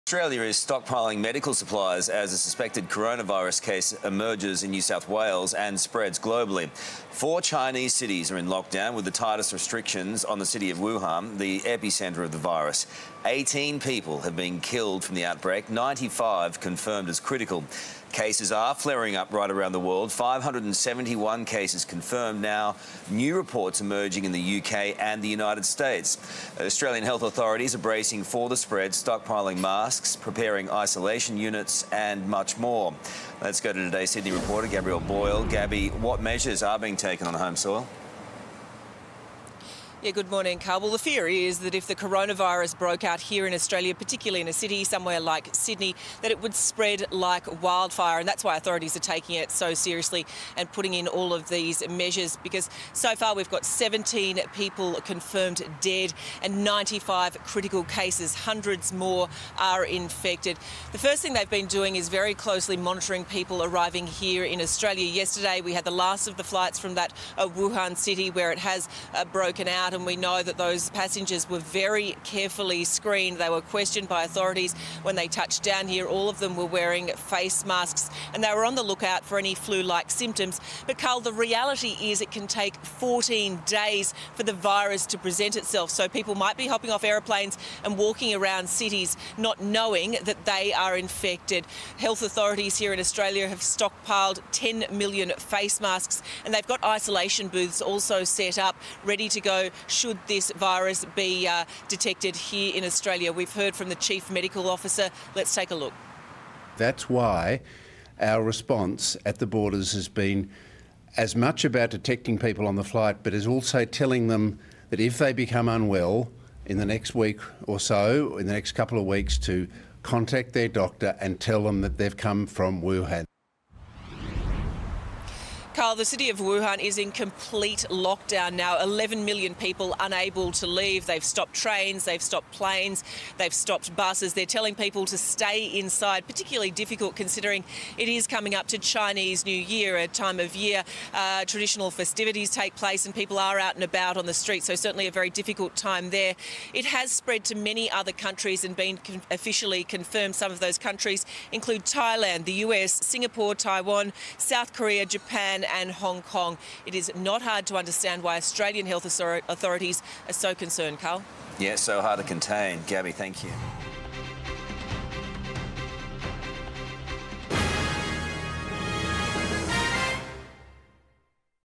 Australia stockpiles medical supplies as coronavirus spreads | Nine News Australia
Australia has started stockpiling medical supplies and readying hospital isolation units as concerns a deadly new coronavirus strain, originating in Wuhan, China, could make it's way here.